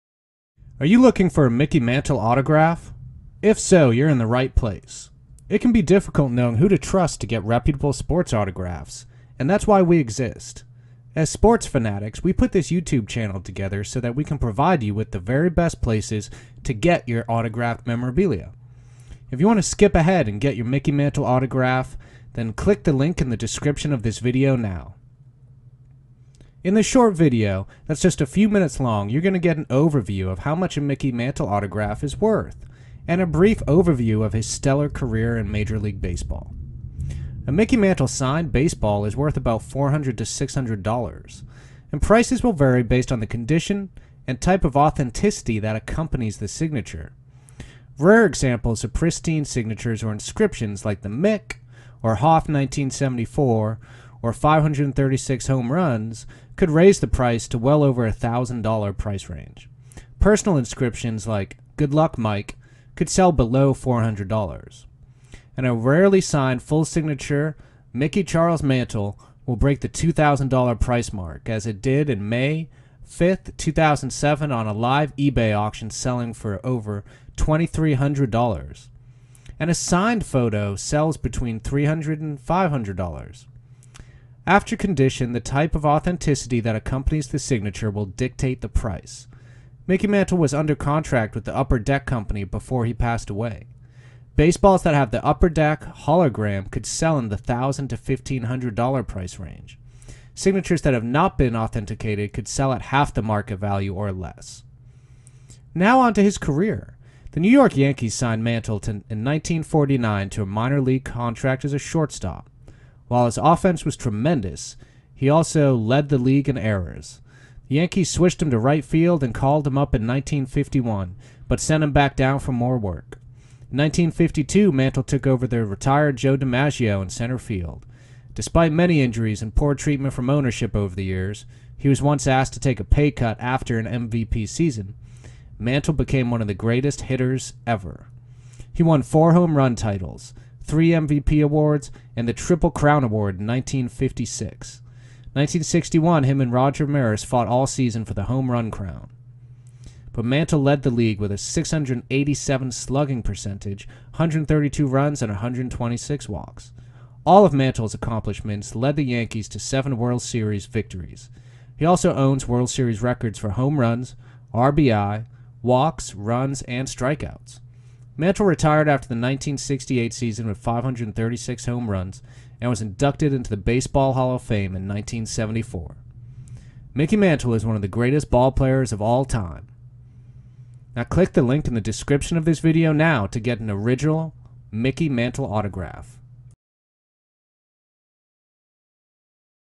Mickey Mantle Autograph - What's Mickey's Signature Worth?
A Mickey Mantle single signed baseball is worth about $400.-$600. Price will vary based on condition, and type of authenticity that accompanies the signature. Rare examples of pristine signatures or inscriptions like "The Mick" "HOF 1974" or 536 HR's" could raise the price to the $1,000. price range. Personal inscription like "Good luck Mike" could sell below $400. A rarely signed full name signature "Mickey Charles Mantle" will break the $2,000 price mark as it did in a May 5th 2007 live eBay auction selling for $2,390. A signed photo sells between $400.-$700.